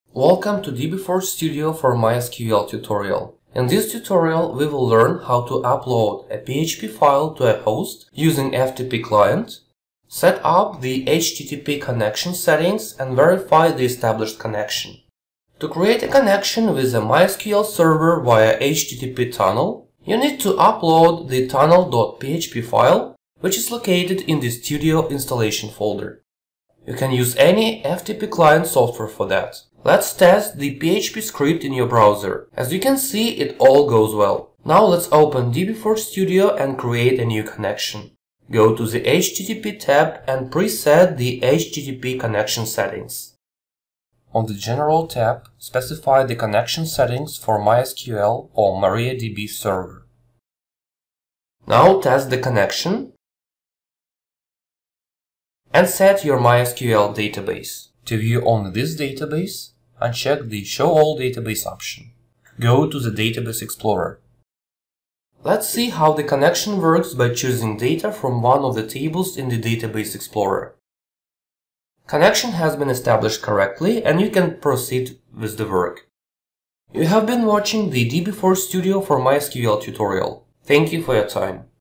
How to Create HTTP Connection using dbForge Studio for MySQL
Learn how to set-up and verify the established connection to MySQL over HTTP tunnel using dbForge Studio for MySQL